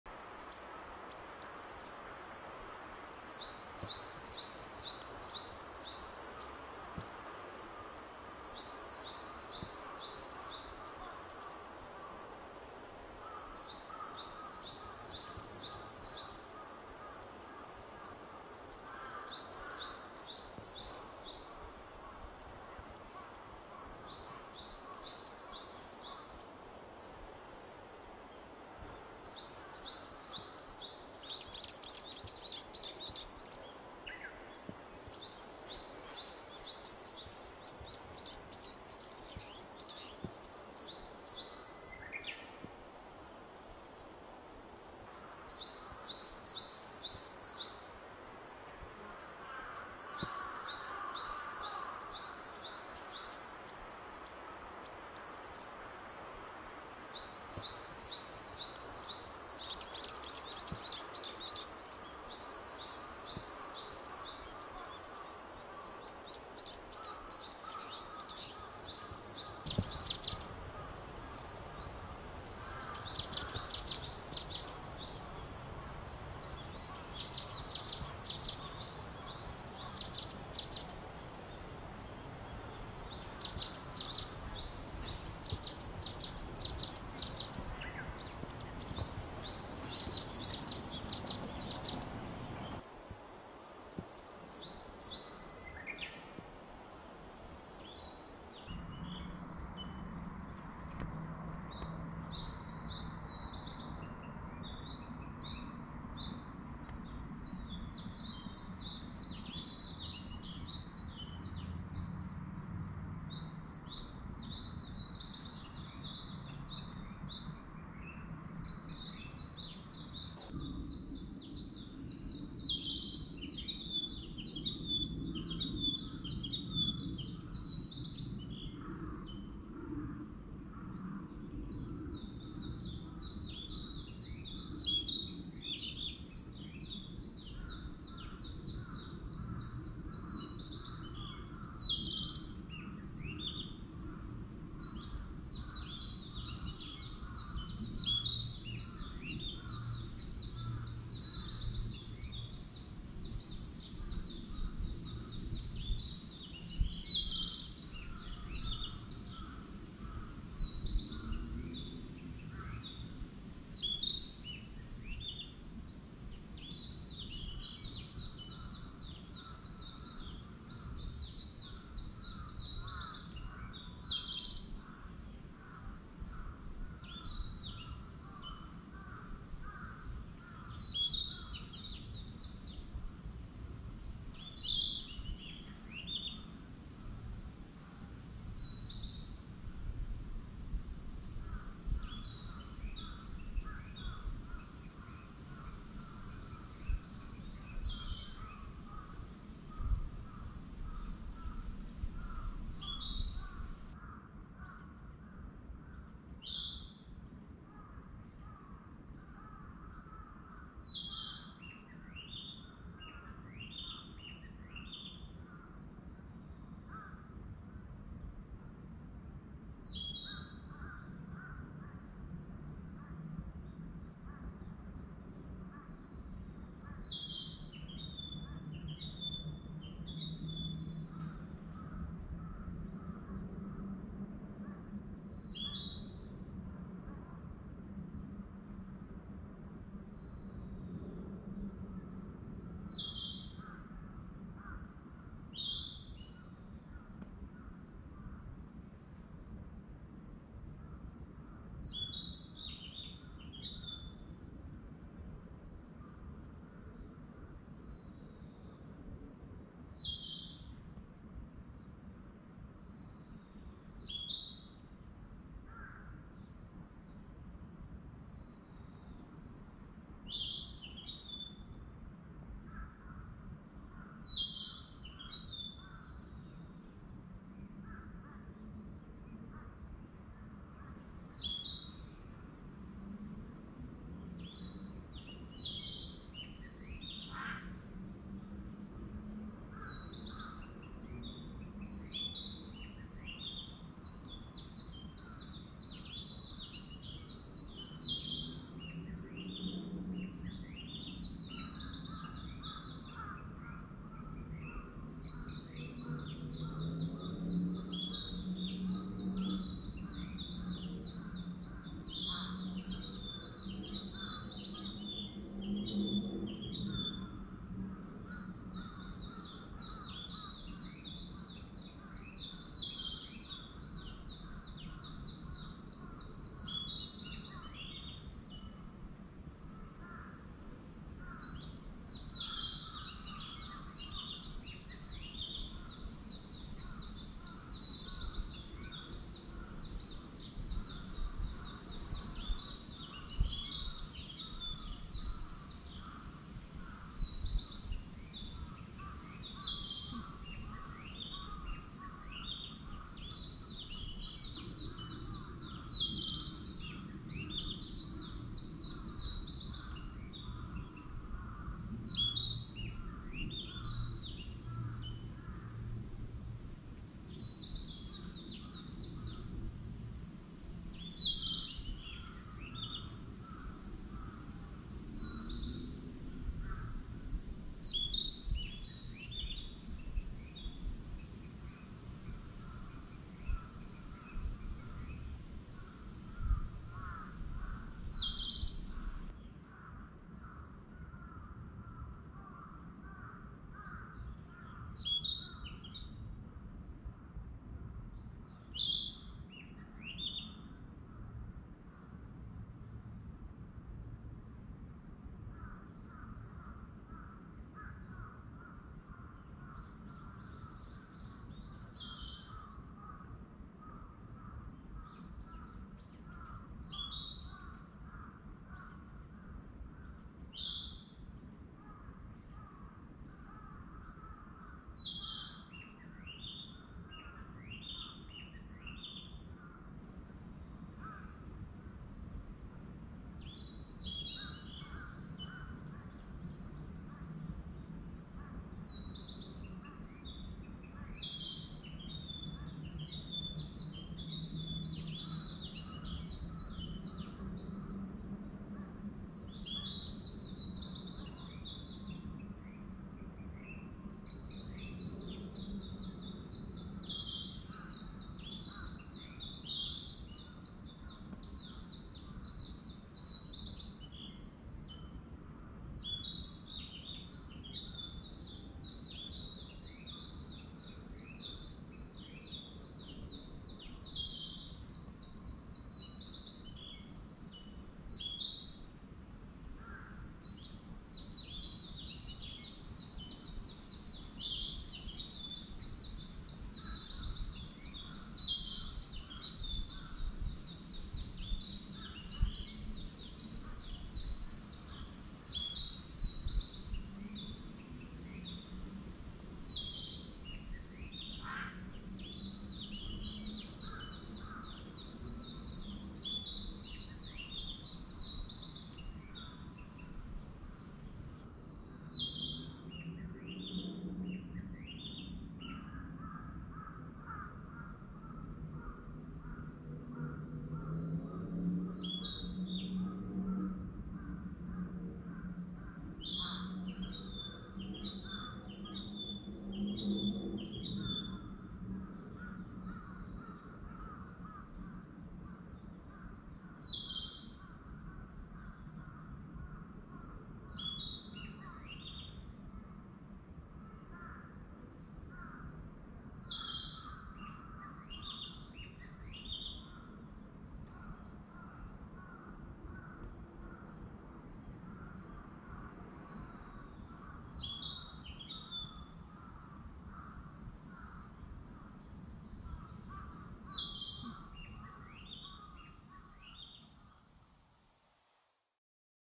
森の木漏れ日 sunlight filtering through trees。
ほの暗い森の中で光のさしている所があります。そこだけキラキラと光っていてとてもきれいです。木洩れ日という言葉は英語にもフランス語にもないそうです。日本語は素晴らしい。There is a place where the sun is shining in the dim forest. Only there is shining and it is very beautiful.